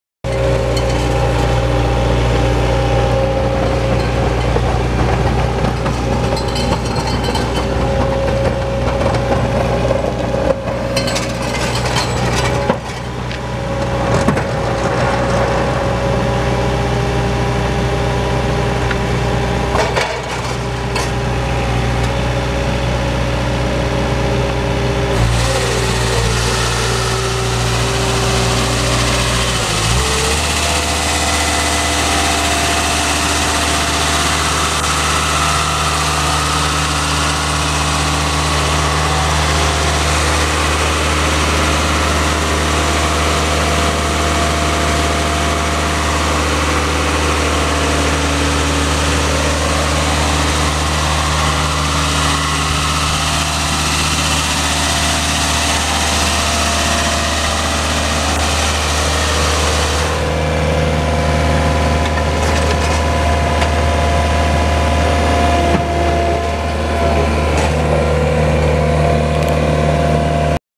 Kelaleikkuri TORO REELMASTER 6500D
Ulkoasultaan siisti iso kelaleikkuri jossa toimii leikkuu, nostot ja moottori